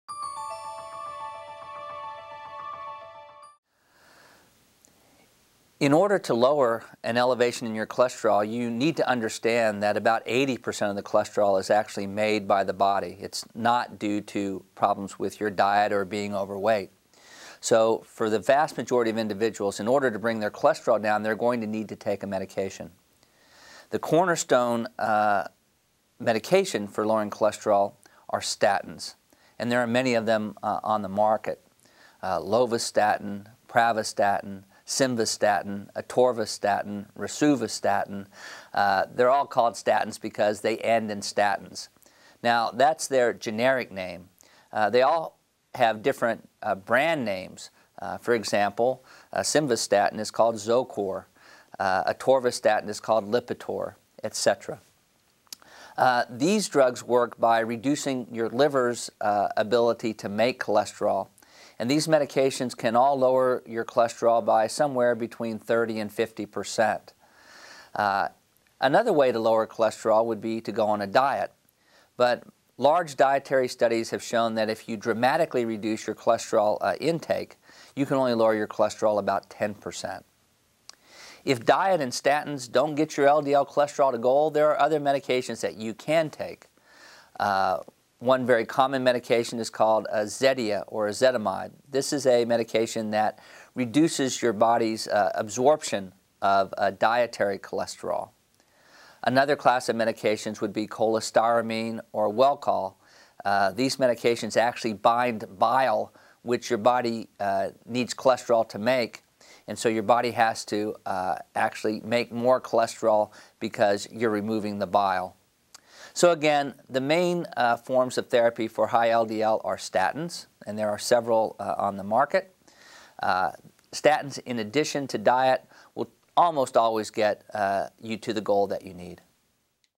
How Can Diabetics Lower LDL Cholesterol?
Learn how to lower LDL cholesterol levels in diabetics. MDs answer your diabetes questions.